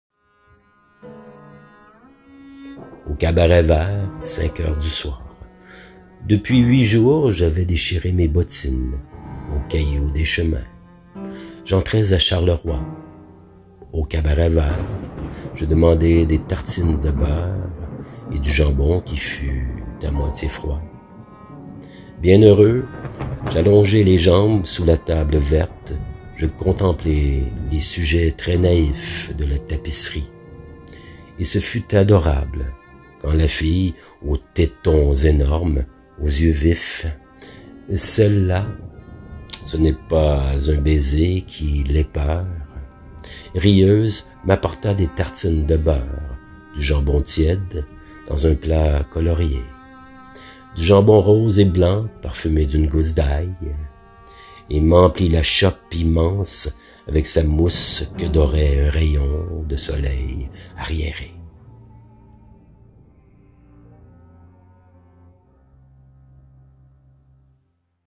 Éric Roger dit Arthur Rimbaud '' AU CABARET-VERT, cinq heures du soir.
tiré de Poésies, Octobre 70
Montage vidéo Yvon Jean